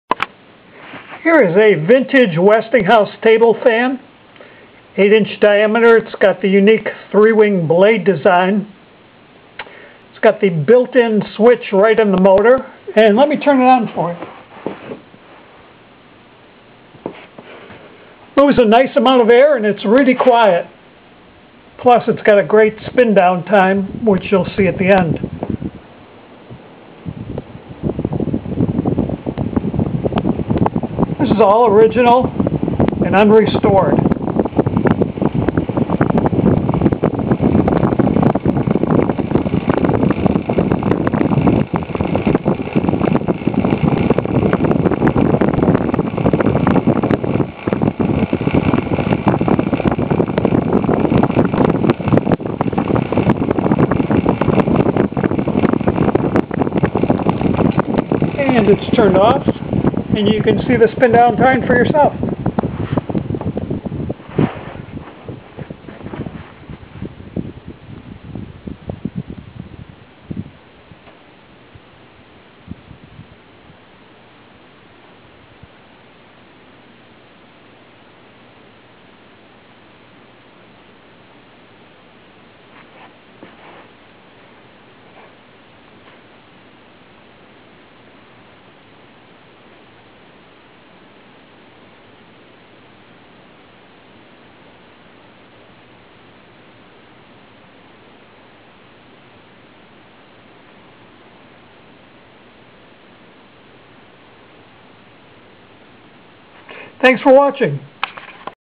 vintage WESTINGHOUSE TABLE FAN, tiny 8" blade
Vintage Westinghouse table fan Style 1229244.
Original and unrestored.  Unique design 3-wing blade, 8" diameter.  Single speed switch built into the motor.  Moves a nice amount of air quietly.  Maybe from the 40s or 50s?  Great bearings.  Watch that spin down time!